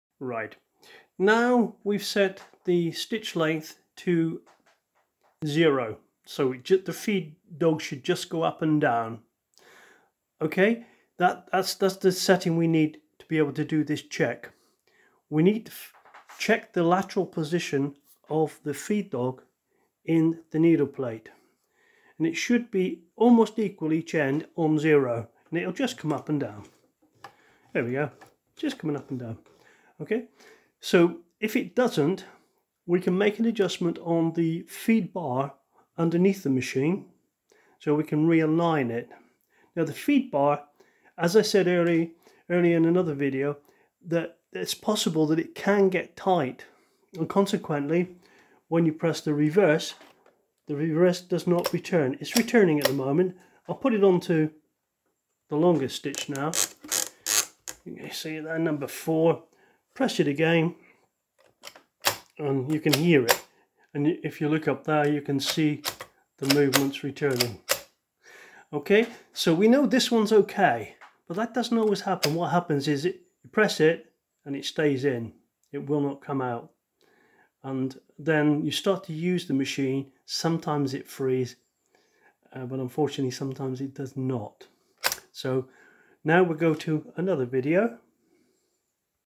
ARTIZAN TRAINING | BROTHER VX FEED DOG LATERAL SETTING
This short video about Feed dog lateral settings and  is 1 in a series of 7 suitable for Brother VX models as well as many styles of older machines using a similar feed drive mechanism.
We run distance learning courses Worldwide for anyone who wishes to understand more about the workings of a sewing machine from the novice to someone who wishes to become a sewing machine technician or mechanic.